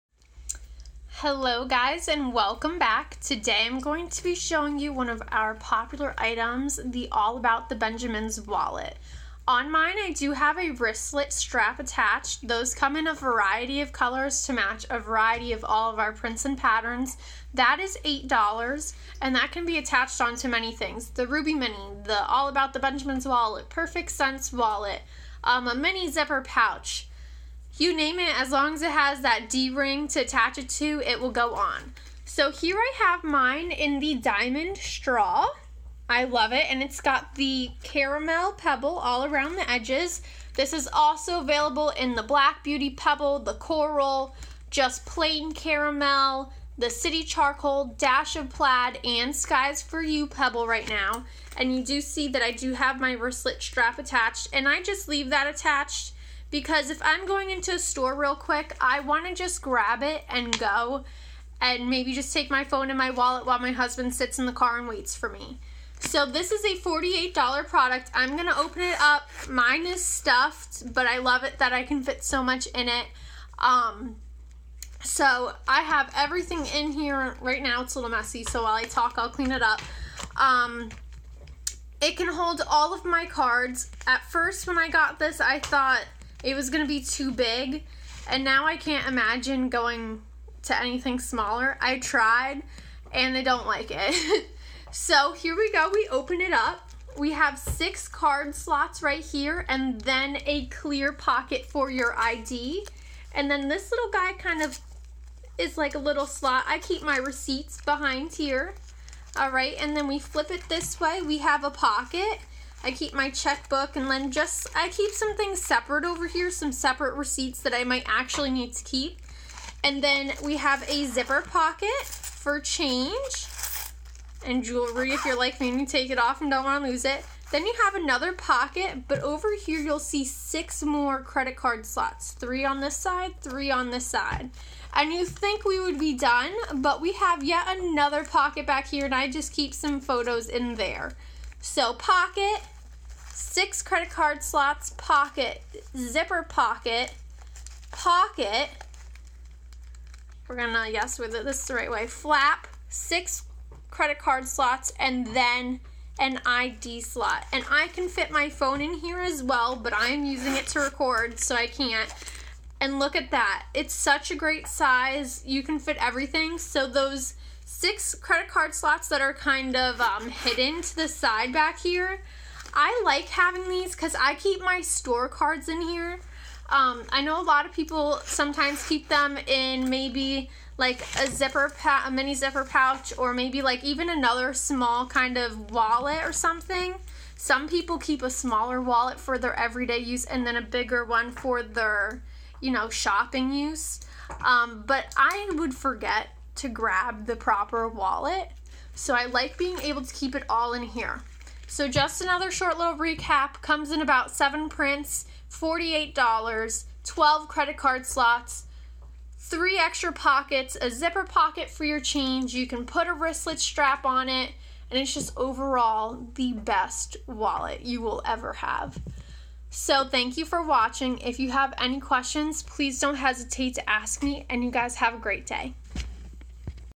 Thirty-one All About The Ben's!
Welcome back guys! Take a look into the All About the Benjamin's with me and fall in love with this stunning and spacious wallet. Have any questions, let me know!